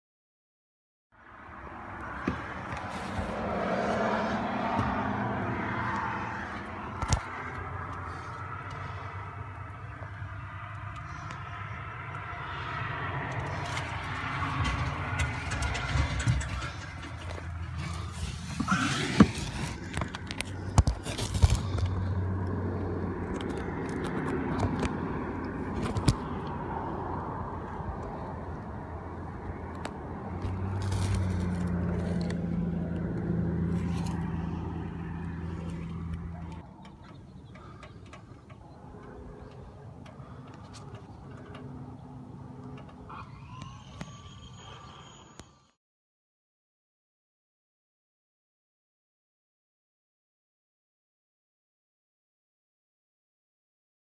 Roof Rat-Removal Inspection at a House in Marietta
We don't use rat poisons inside homes (and rarely use them anywhere else), so every rat-control job we do begins with a thorough inspection. Here's Brad inspecting for rat entry points at a house in Marietta. He also shot the video.